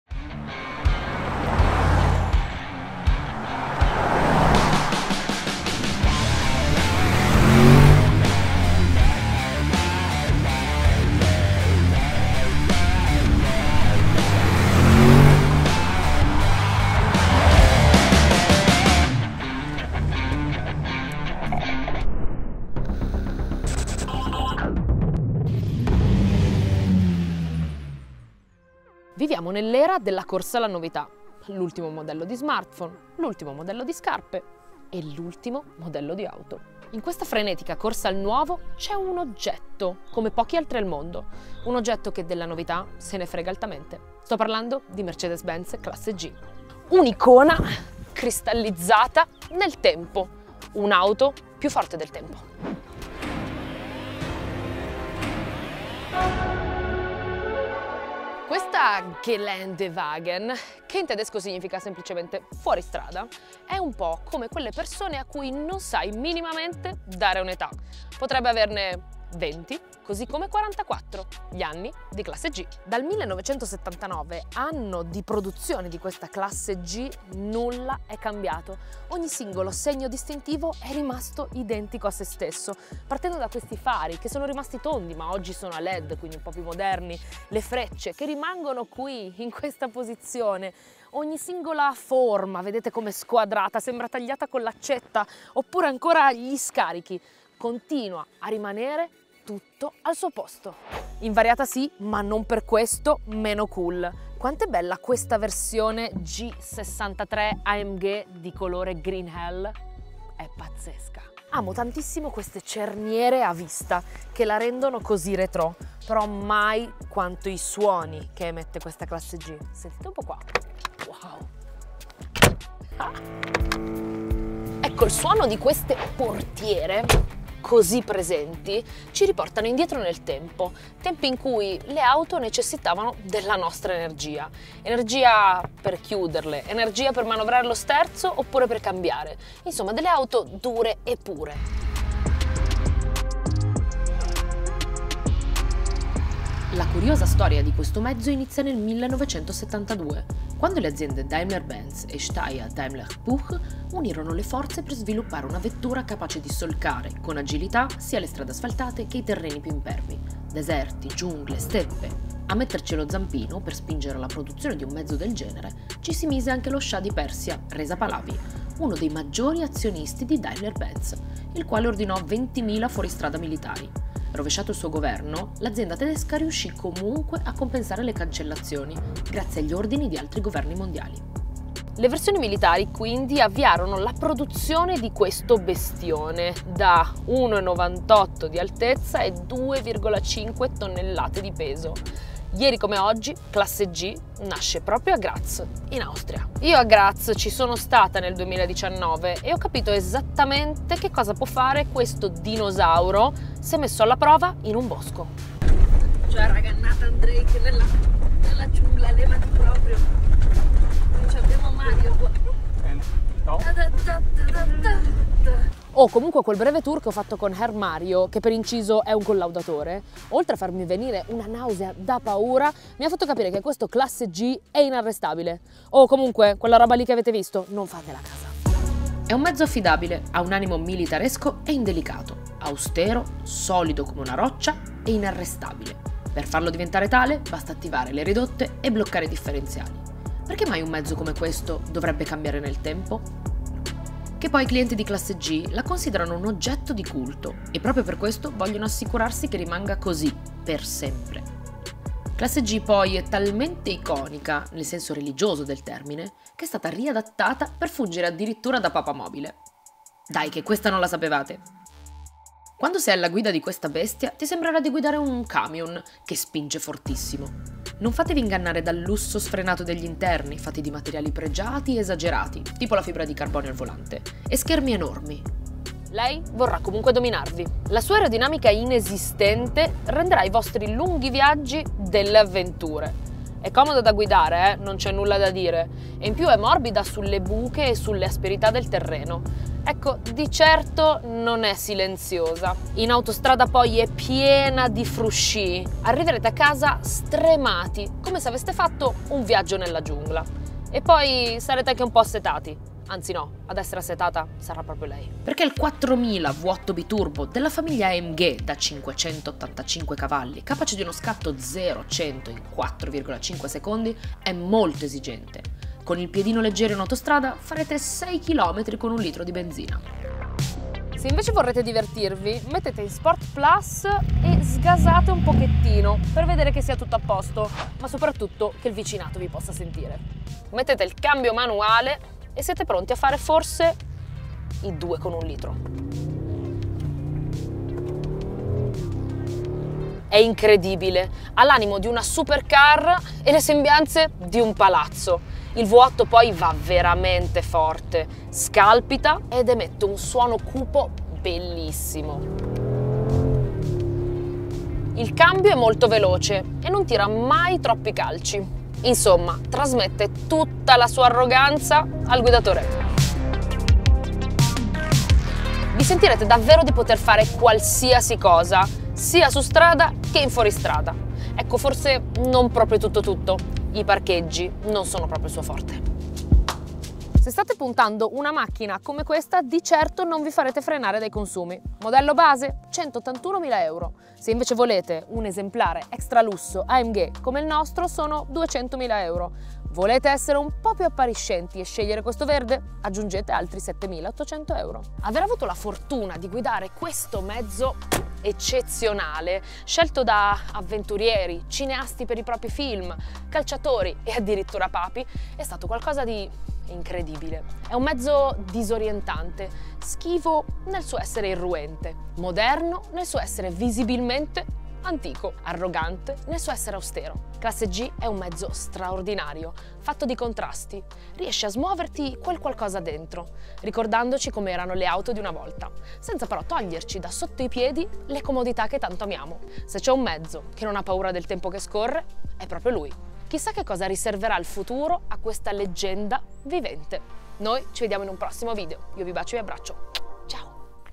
Classe G63 AMG... dopo 40 anni è SEMPRE UGUALE! 💚
Se c'è un'auto che ha ancora davvero qualcosa da dire... questa è MERCEDES CLASSE G... la versione 2023 è rimasta praticamente immutata nel tempo. Non potete immaginare che mezzo straordinario ho avuto tra le mani. Vi racconto tutto in questo test drive!